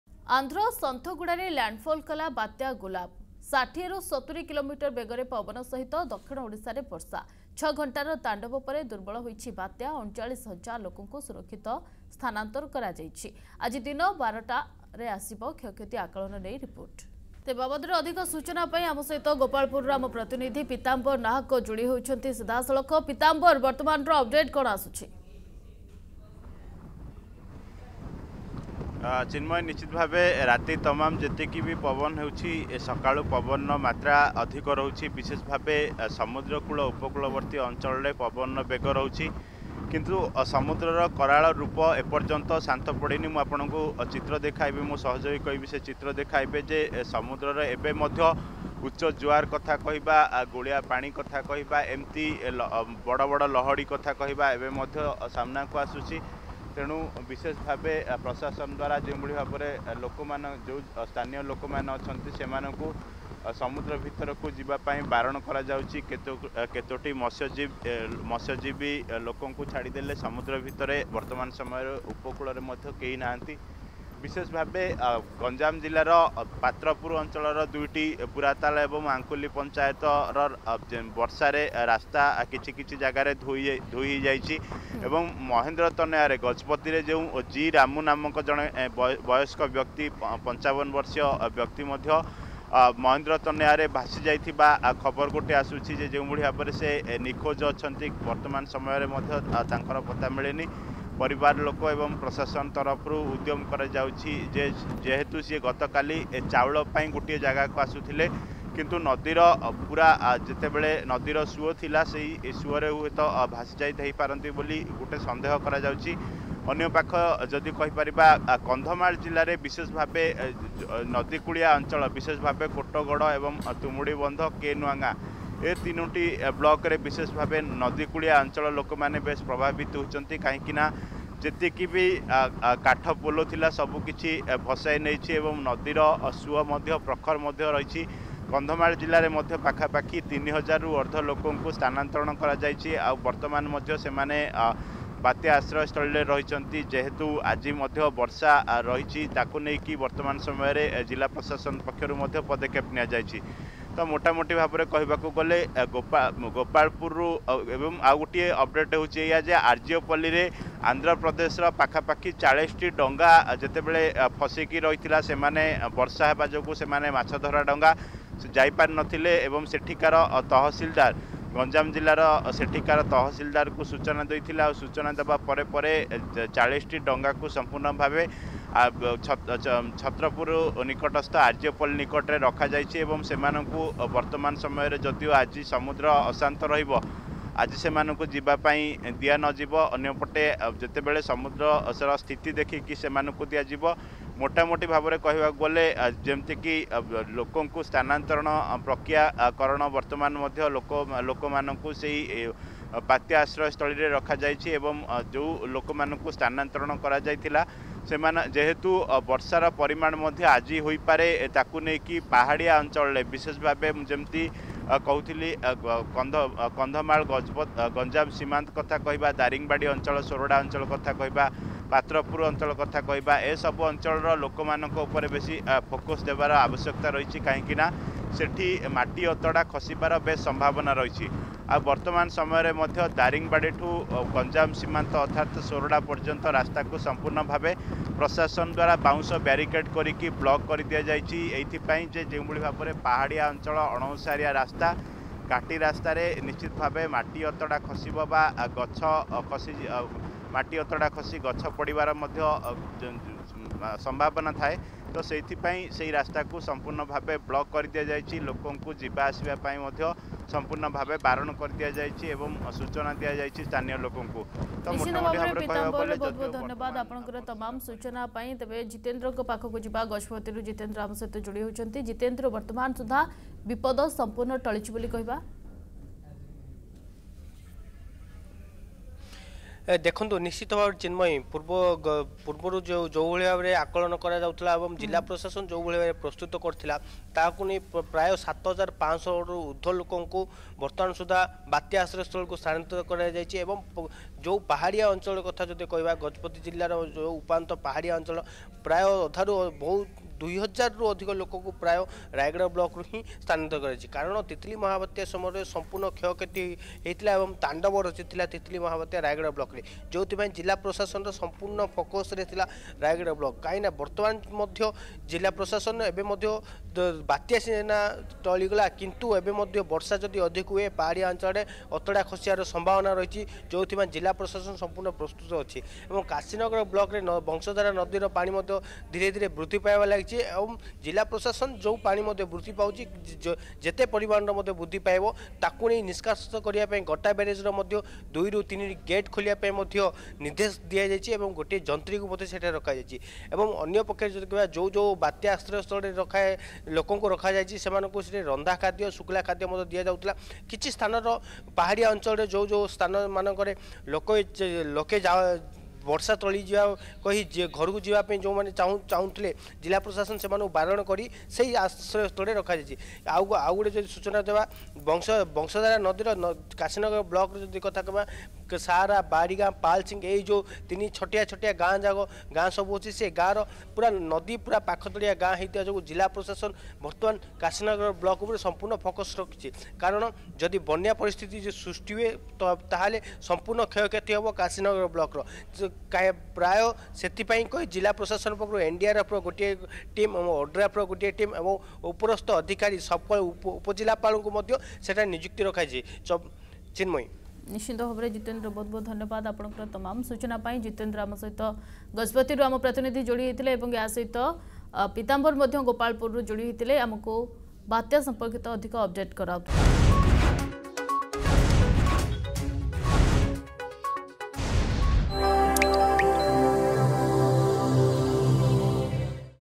ଲ୍ୟାଣ୍ଡଫଲ କଲା ବାତ୍ୟା ଗୁଲାବ ଆନ୍ଧ୍ର ସନ୍ଥଗୁଡାରେ
ଆନ୍ଧ୍ର ସନ୍ଥଗୁଡାରେ ଲ୍ୟାଣ୍ଡଫଲ କଲା ବାତ୍ୟା ଗୁଲାବ। 6ଘଣ୍ଟାର ତାଣ୍ଡବ ପରେ ଦୁର୍ବଳ ହେଲା ବାତ୍ୟା। ବାତ୍ୟା ଗୁଲାବ୍‌ର ଲ୍ୟାଣ୍ଡଫଲ୍ ପ୍ରକ୍ରିୟା ସରିଥିଲେ ମଧ୍ୟ ଏବେସୁଦ୍ଧା କୌଣସି ବିଶେଷ କ୍ଷୟକ୍ଷତି ସୂଚନା ମିଳିନାହିଁ।